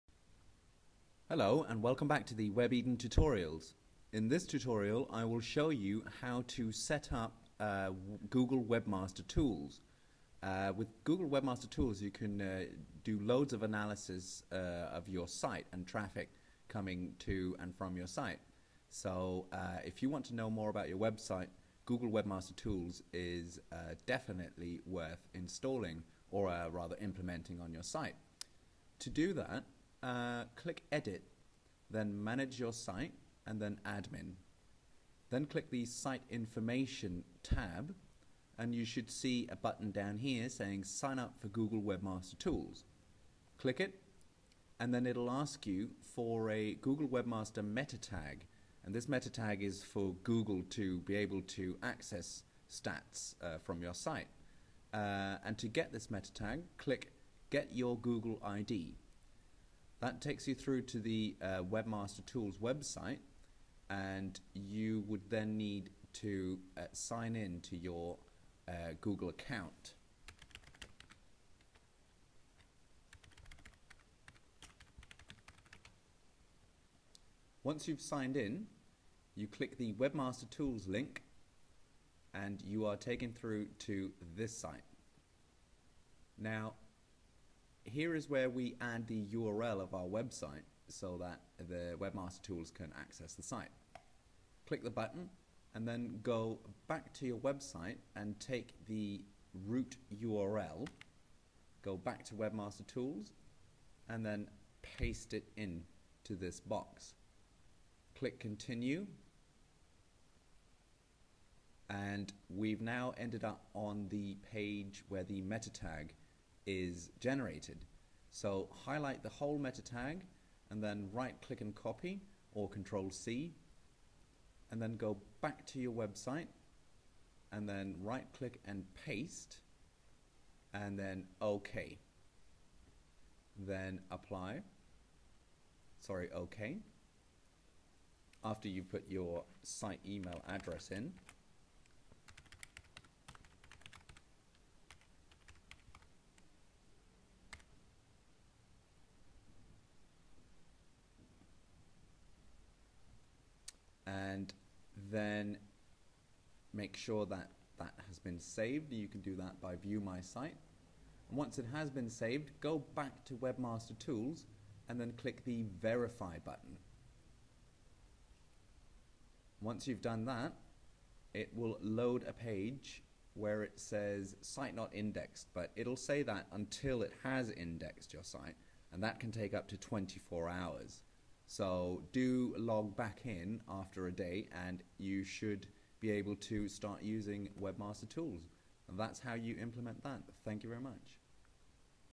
Implementing Google Web Master Tools: WebEden.co.uk Free Website Builder
Webeden is an online website building service. This is a tutorial showing you how to implement the Google Web Master Tools.